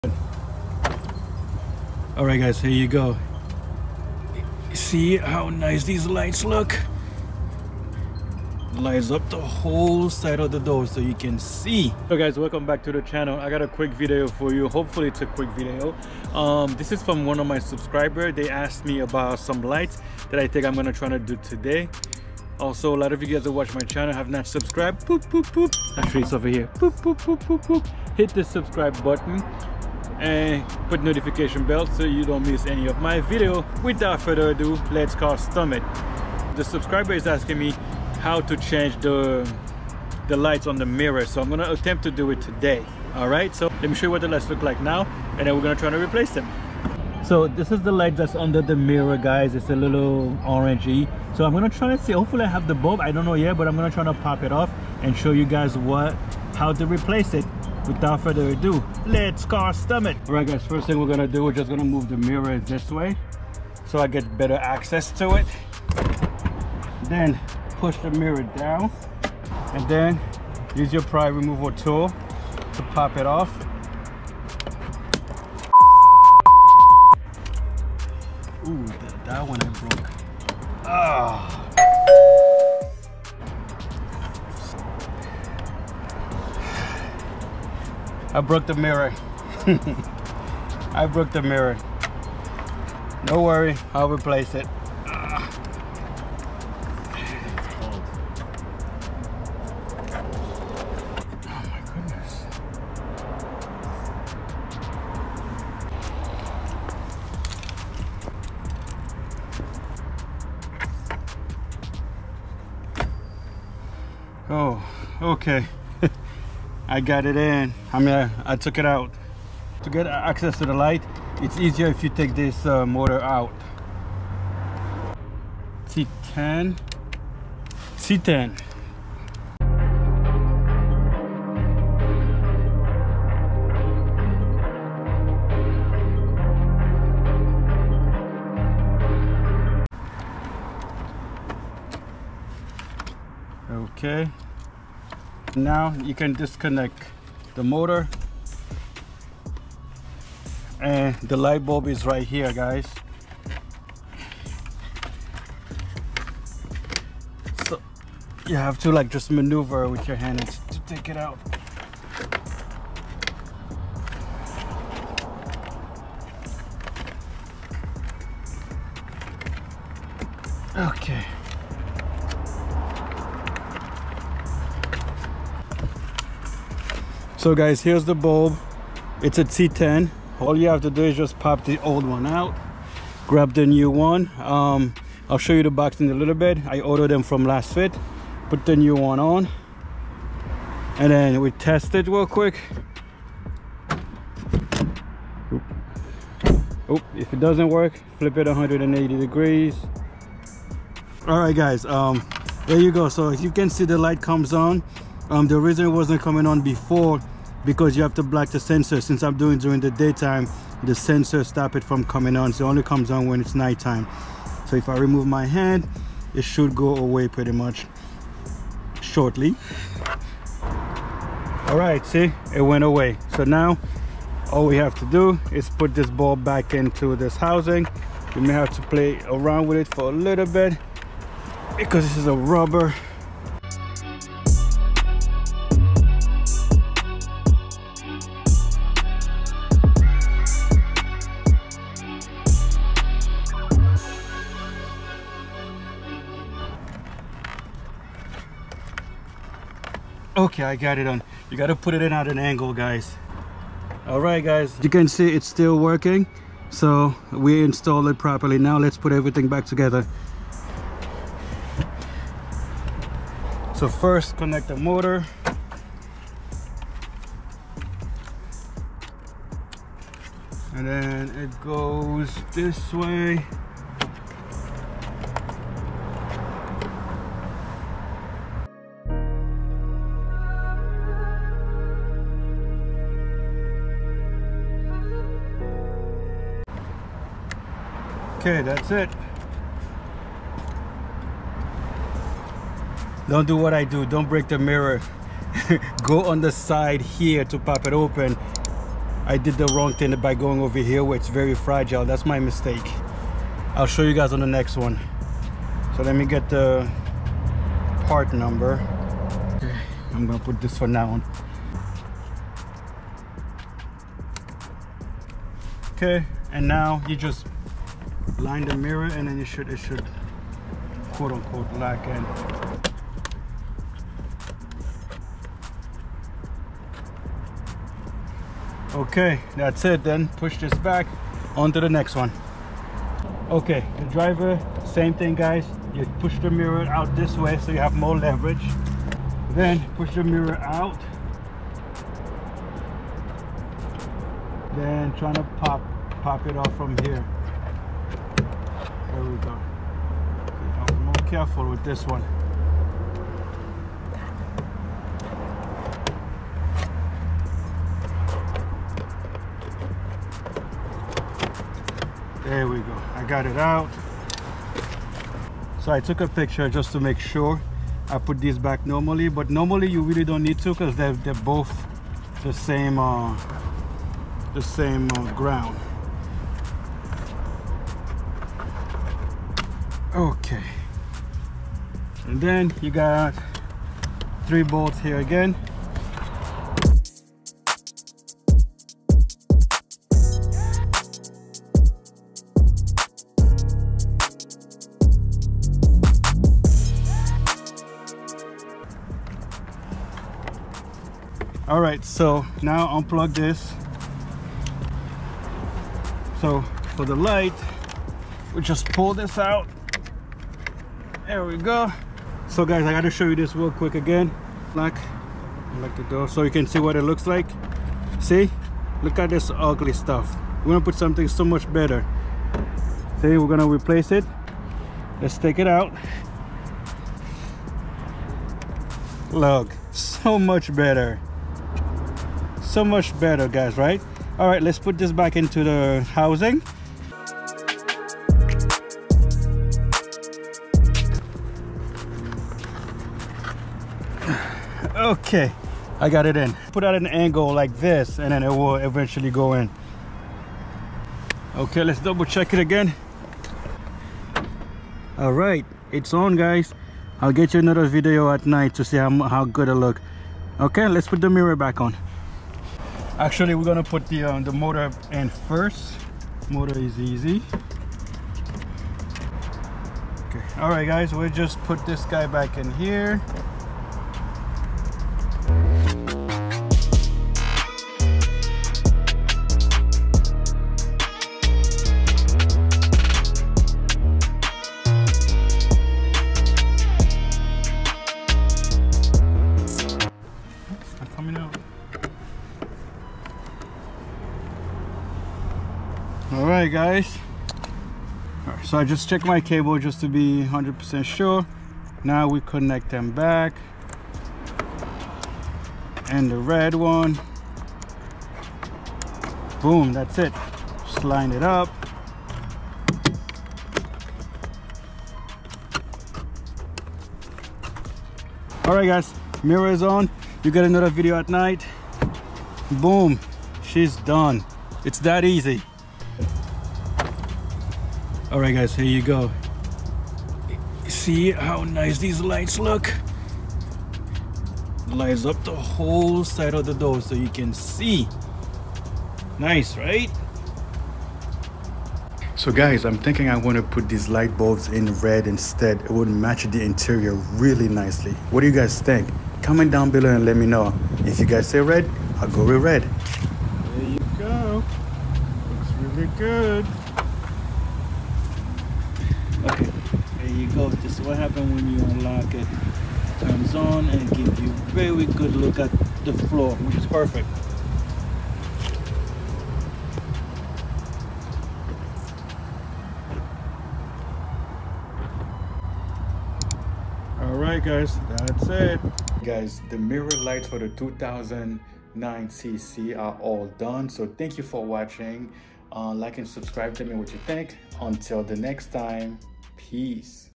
2009 VW CC Mirror Approach LED light upgrade
In This videos I show you guys how to upgrade you VW CC mirror approach lights to LED. This mirror lights should work for VW CC from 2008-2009 until 2012  CC model. when you are taking out the mirror  please BE SUPPER CAREFULL,  SO YOU DONT BRAKE IT. this will  elevate your lights and make your car look so much nicer.

You can buy the LED bulb from Lasfit

The bulbs Side is T10